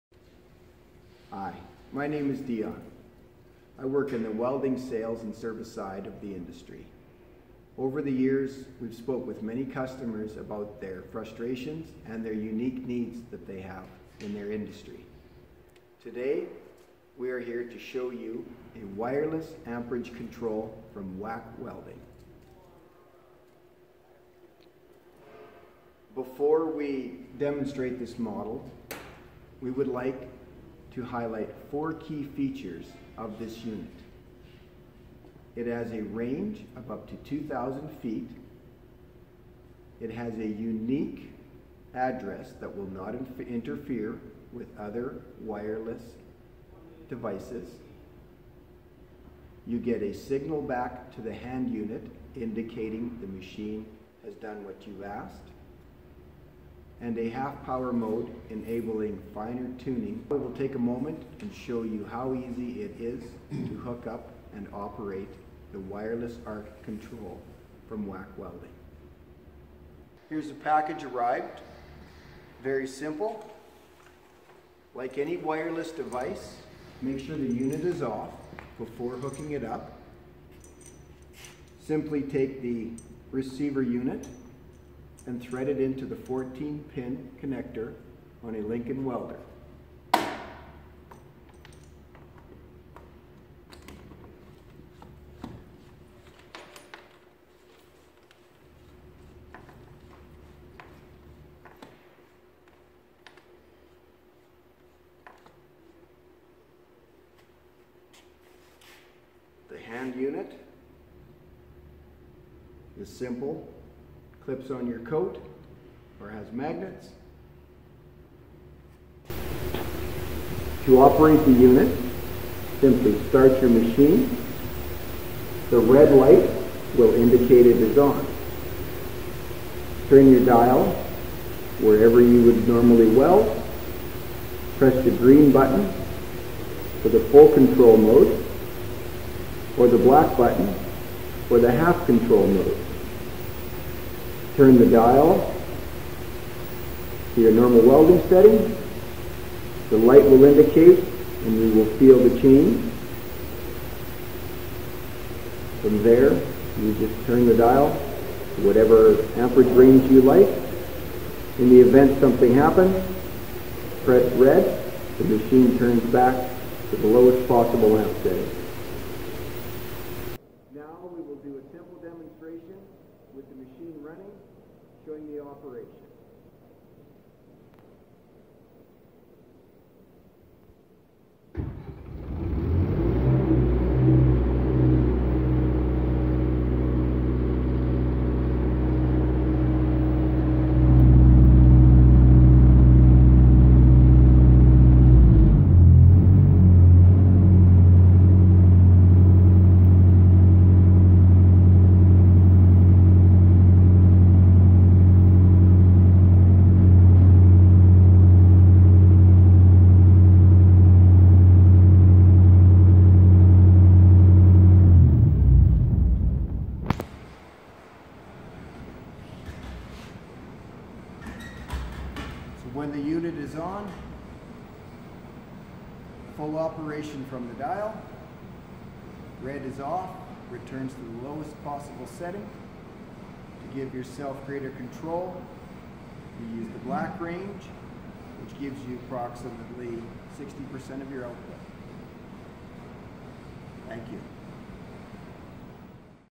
Wireless Amperage Control for Welding Units
Watch how the WAC wireless Remote Control lets you vary your welding unit’s amperage from 2000 feet.

You can change your amperage by just turning a dial without pressing any buttons plus you get immediate acknowledgement of your change.